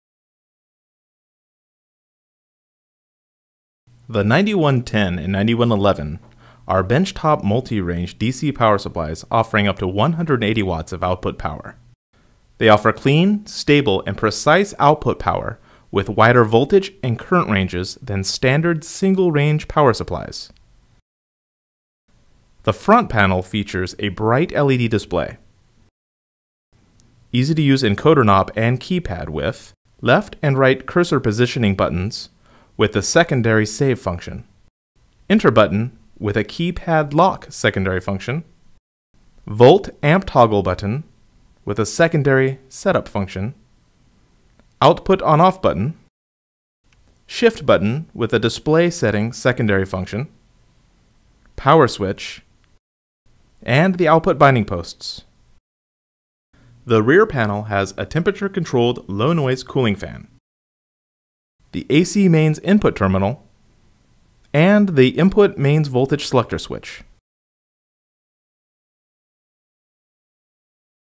9110 Series - 60 V Multi Range DC Power Supplies
B&K Precision's 9110 Series multi-ranging DC power supplies are compact, with output ratings up to 180 W within 60 V/ 8 A (model 9111) or 100 W within 60 V/ 5 A (model 9110). Unlike conventional power supplies with fixed output ratings, they automatically recalculate voltage and current limits for each setting, providing any Volt/Amp combination within the rated power.